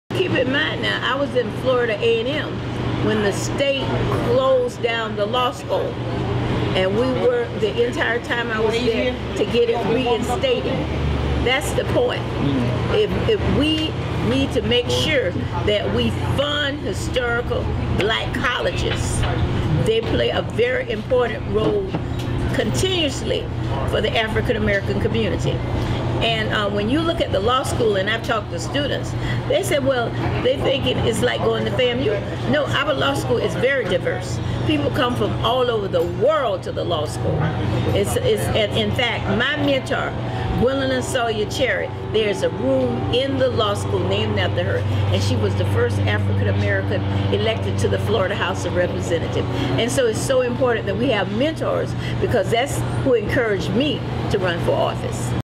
Congresswoman Brown fights for HBCU
Congresswoman Corrine Brown discusses the importance of supporting Historically Black Colleges and Universities.